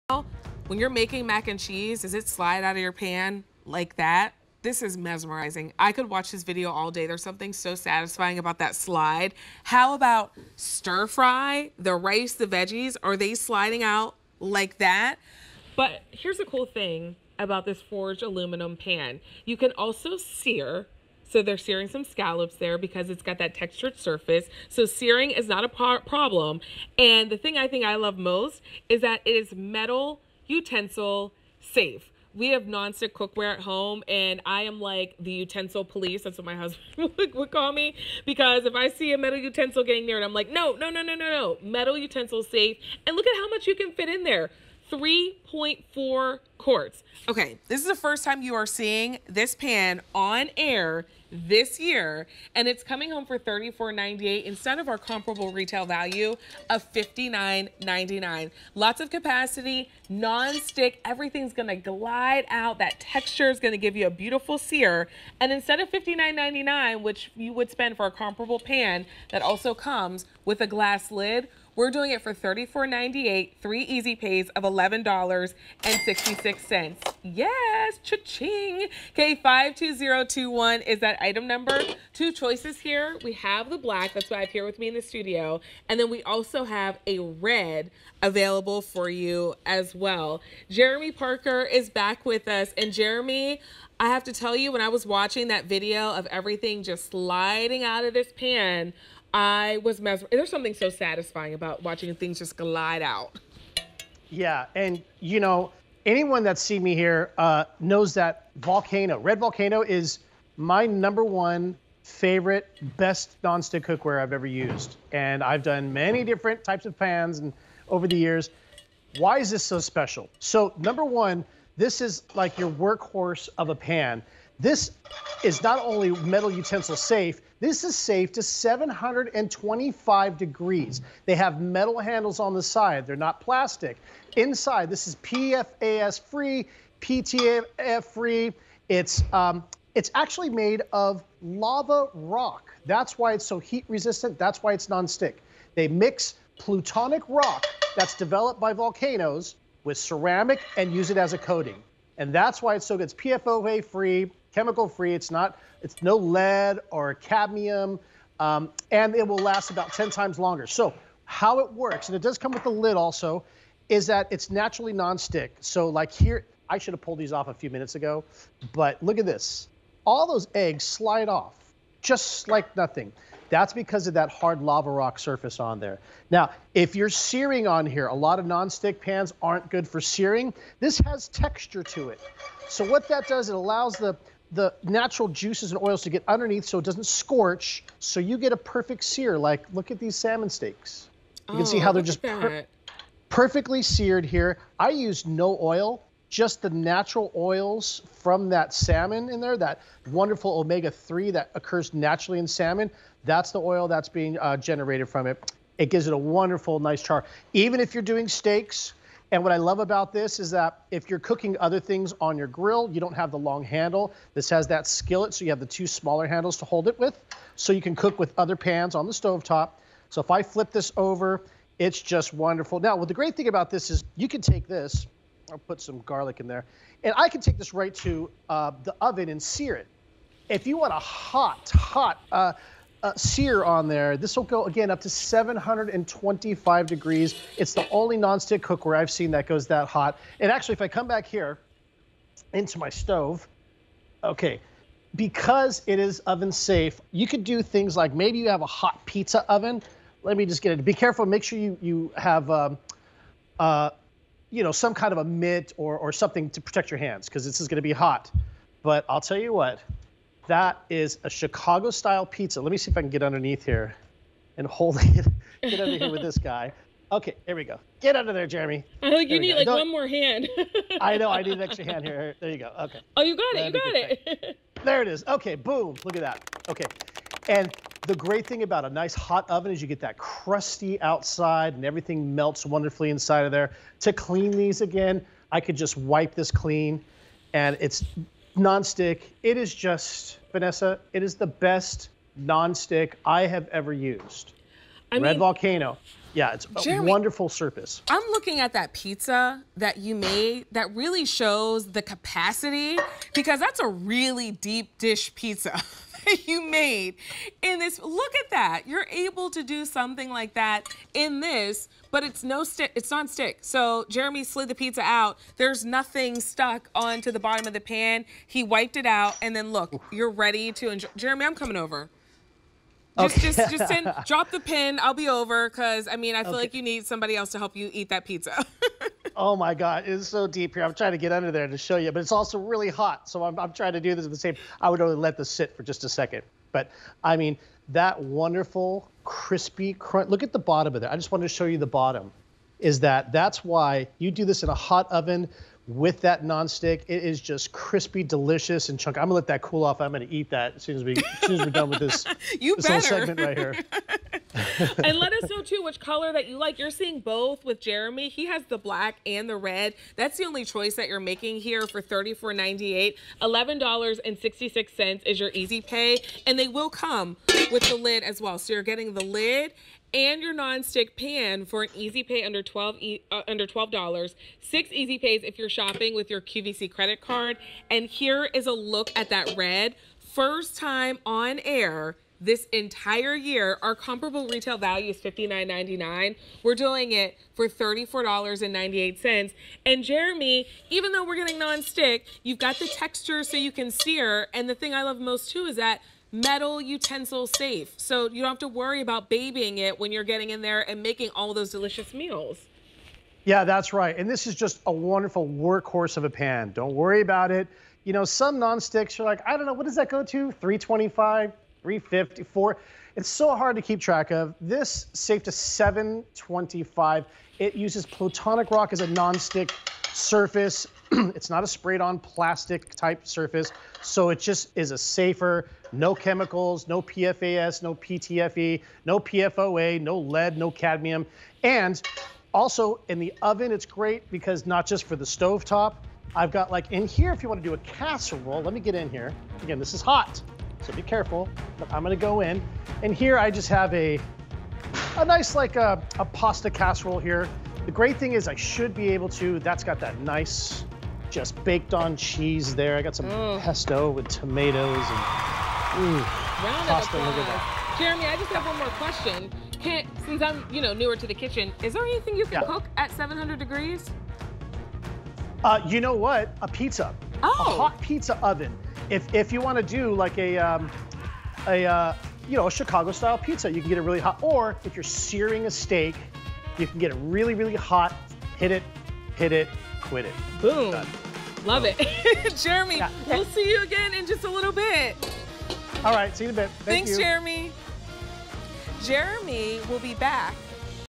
Red Volcano 11" Forged Aluminum Covered Everyday Pan on QVC
Red Volcano 11" Forged Aluminum Covered Everyday Pan

Has your cooking game gotten stronger? (We're betting yes.) Reward your new culinary confidence with an everyday pan built to be as strong as your mad skills. Infused with plutonic rock, this Red Volcano covered pan is engineered for durability and offered in a must-have size every home chef needs. So, whether you've mastered porterhouse steak with an herb-infused compound butter or just experimenting with the latest recipe blowing up your socials, this everyday pan is with you all the way. From Red Volcano.